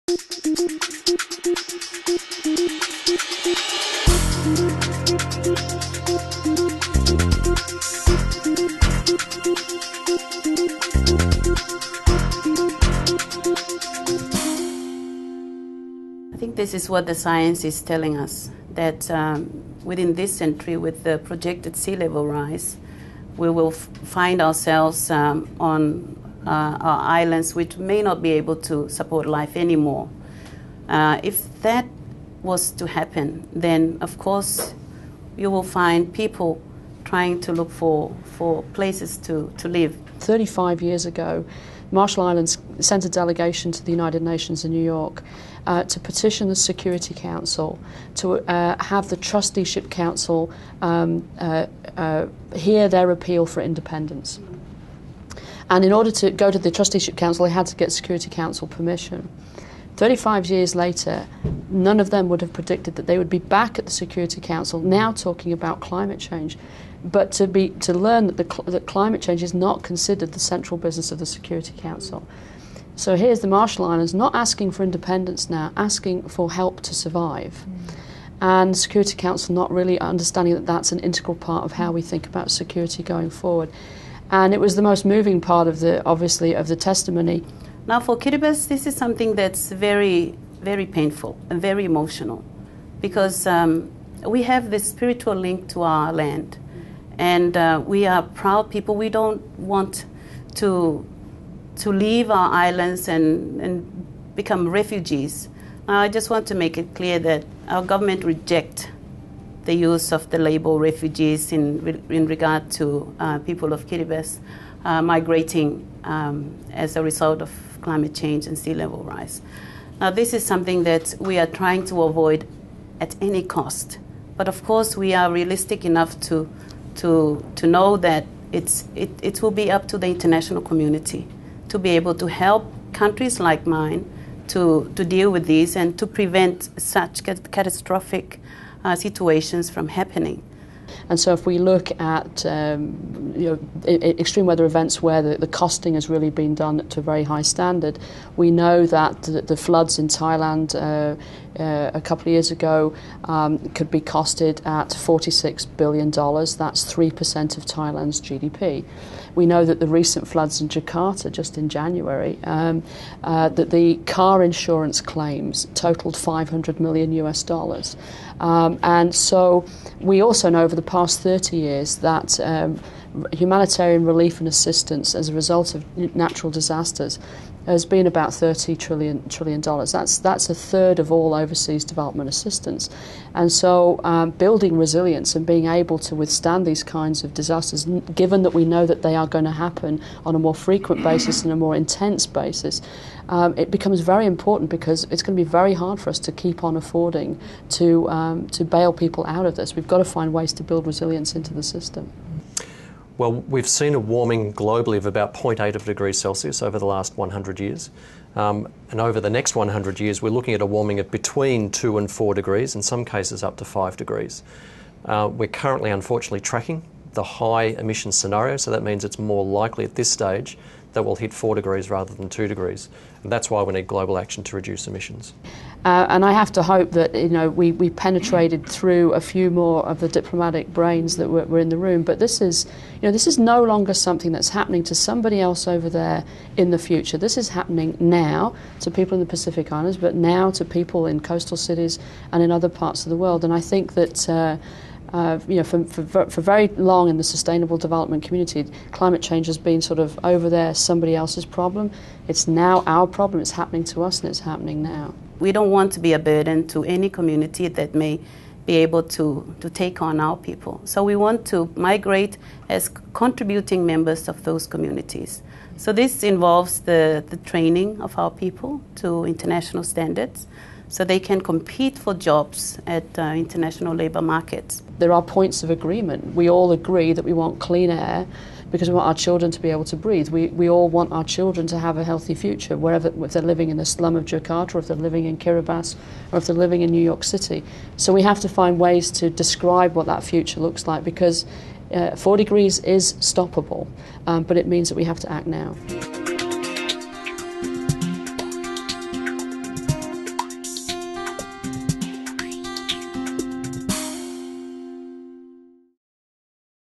Praxis Discussion Series - Climate Change (Short Version)
A panel discussion on avoiding a 4 degree warmer world with Rachel Kyte, World Bank Vice President for Sustainable Development, Tessie Eria Lambourne, Secretary of Foreign Affairs and Immigration for the Government of Kiribati, and Kevin Hennessy, Principal Research Scientist at CSIRO Marine and Atmospheric Research .
The World Bank has warned that the world is heading towards a 4-degree warmer world by the end of the century, which would trigger violent changes like extreme heat-waves, declining food stocks, more intense natural disasters, and a sea-level rise that would directly affect hundreds of millions of people. This is not inevitable, so what needs be done to avoid a drastically warming world?    How can small island states, like those in the Pacific, adapt to the challenges of climate change, and address the risks they face?

This event is part of the Praxis Discussion Series 2013, connecting to Papua New Guinea, Solomon Islands and Timor-Leste.

Recorded:   Friday, March 22, 1pm -- 2pm in Sydney, NSW
Moderator: Olivia Rousset, journalist